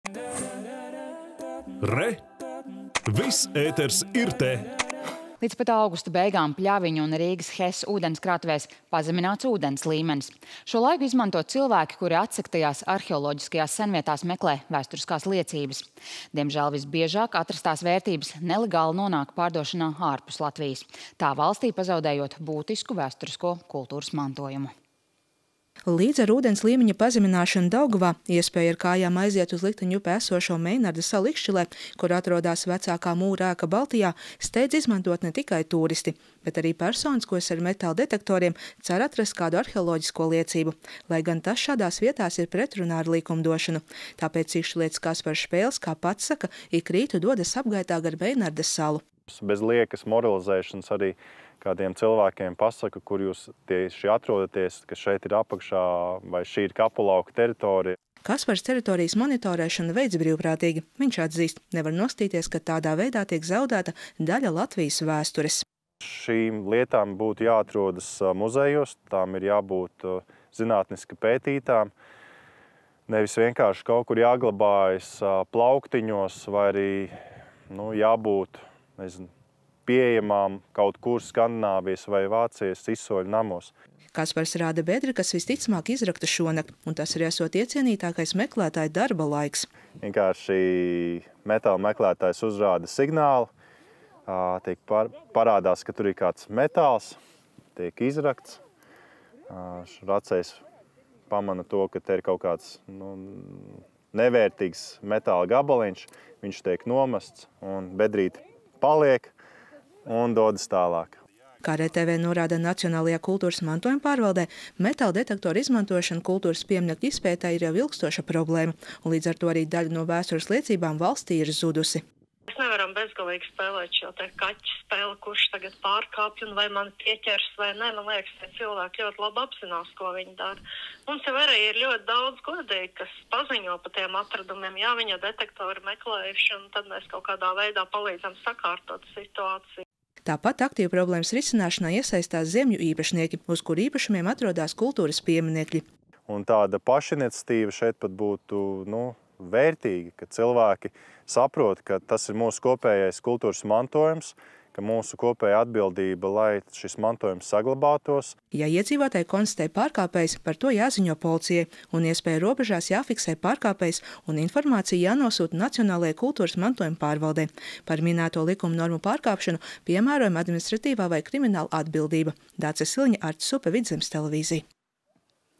Meinarda salā nelegāli meklē senlietas
Līdz pat augusta beigām Pļaviņu un Rīgas HES ūdenskrātuvēs pazemināts ūdens līmenis. Šo laiku aktīvi izmanto personas, kuras atsegtajās arheoloģiskajās senvietās meklē vēsturiskās liecības, kas gan visbiežāk nelegāli tālāk nonāk pārdošanā ārpus Latvijas un tā valstij pazaudējot būtisku vēsturisko kultūras mantojumu.